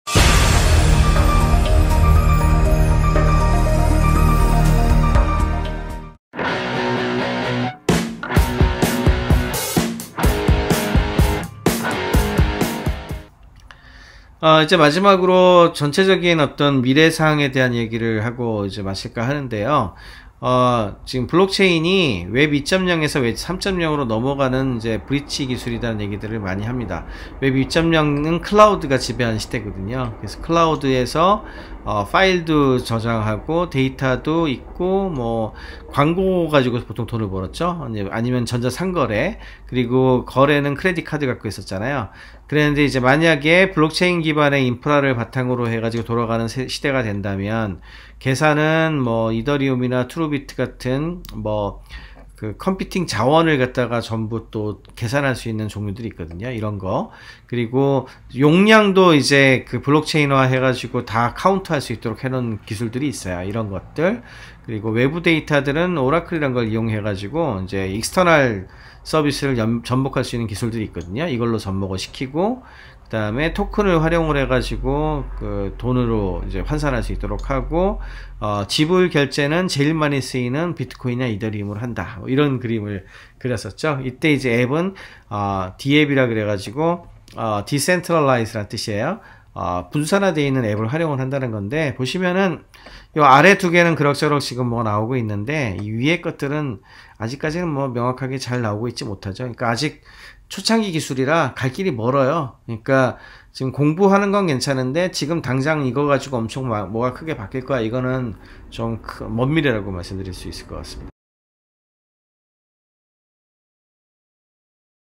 웹 2.0 이후, 웹 3.0 시대의 블록체인의 역할
블록체인 미니렉처 시리즈. 이번 시간에는 웹 2.0 이후, 거래의 인프라가 적용된 다음 세대의 웹인 웹 3.0 의 시대가 된다면 어떤 구조나 기술이 중요할지? 그리고 블록체인 기술이 이런 변화에 어떤 영향을 미칠 수 있을지에 대해 알아보도록 하겠습니다.